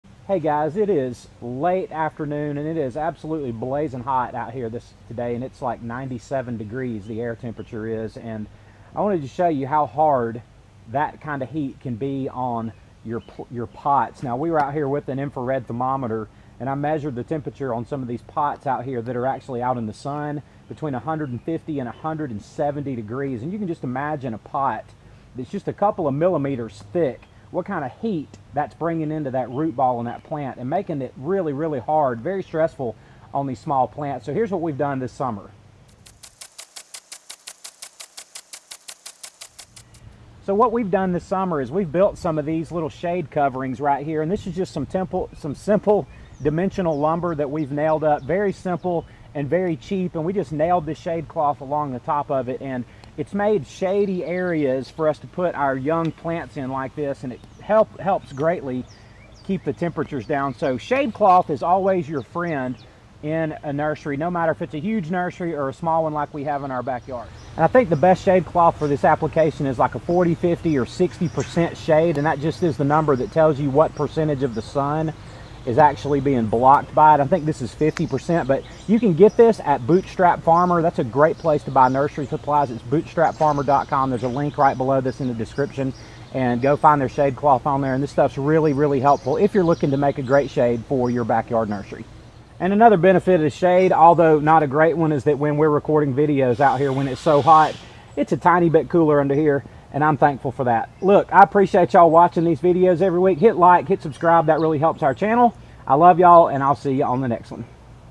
BEAT the SUMMER HEAT in your nursery using SHADE CLOTH!!
Shade cloth is an extremely valuable part of summer success in a small nursery!

Corona Clippers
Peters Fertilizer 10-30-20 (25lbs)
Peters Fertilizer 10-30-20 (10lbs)
Jacks Plant Starter (25lbs)
Dramm Irrigation Wand
Flexzilla Hose